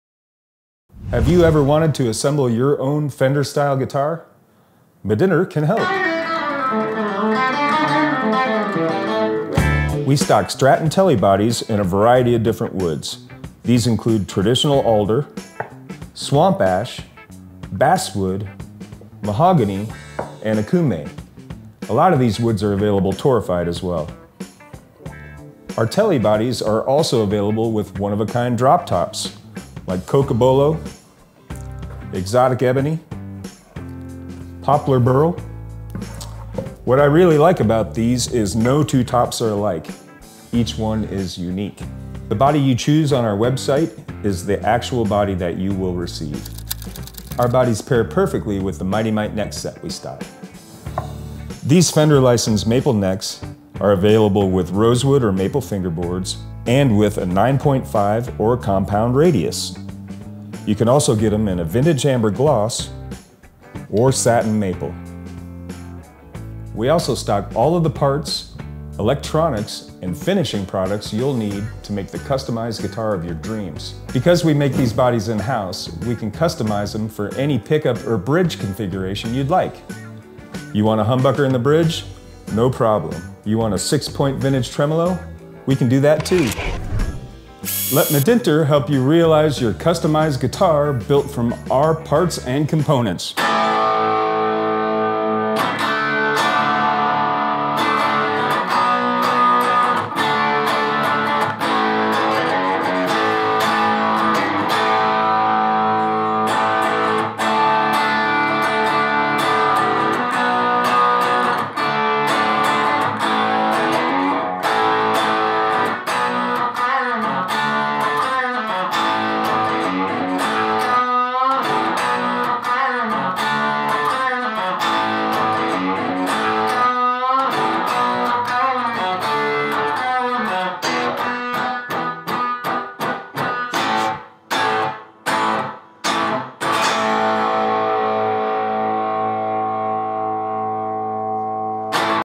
The Easy Way to Build Your Dream Guitar
If you’ve ever wanted to build your dream guitar but weren’t sure where to start we have the perfect solution.  With our custom made electric guitar bodies and Mighty Mite necks you can build a custom guitar yourself.  No expensive tools and no workshop needed.